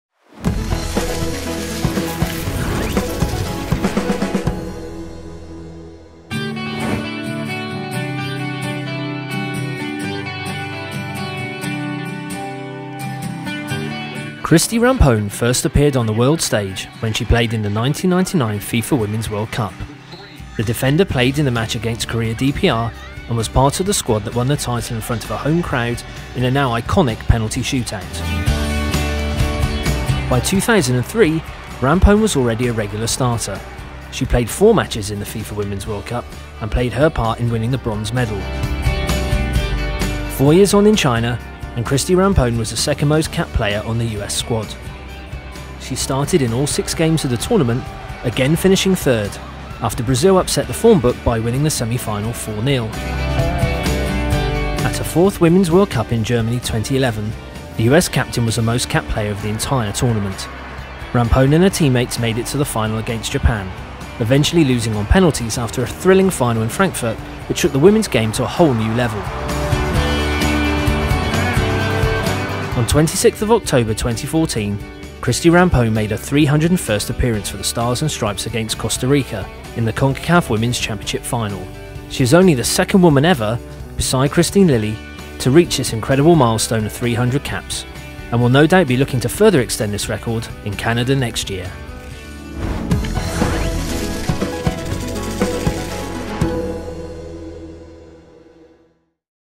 Rampone: A career in World Cups
U.S. captain Christie Rampone played her 301st cap for the USA in the final of the Women's CONCACAF Championship 2014 on October 26. FIFA.com and FIFA on YouTube look back on her four FIFA Women's World Cups between 1999 and 2011.